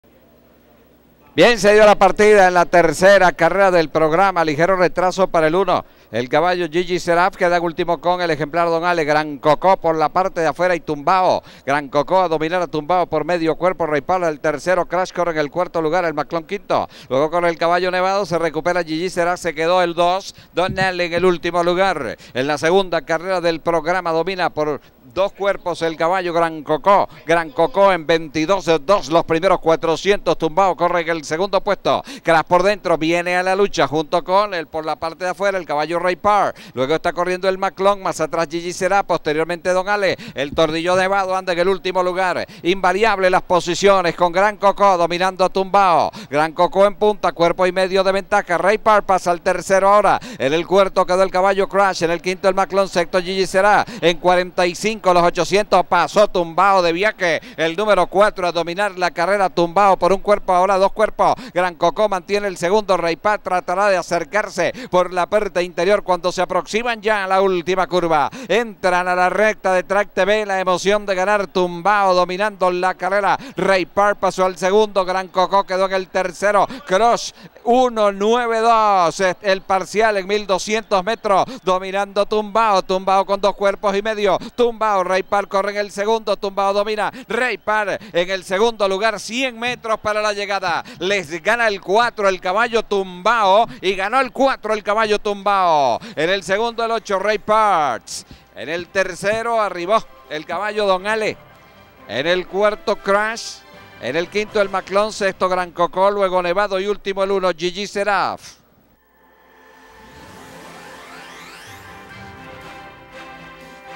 🔅 Carrera Comun 2009 TUMBAO 🔅
Se dio la partida y salen Gran Coco con Tumbao a luchar por el primer puesto, Rey Parts, El Maclon, Don Ale, Crash y Gigi Seraf, cuando entran en la recta final, ya Tumbao pasa a dominar y ganar la prueba, luego llegaron: Rey Parts, Don Ale, Crash y cierra El Maclon

Jinete: S. Gonzalez
Entrena: Gustavo Delgado
Narra: A. Tarkani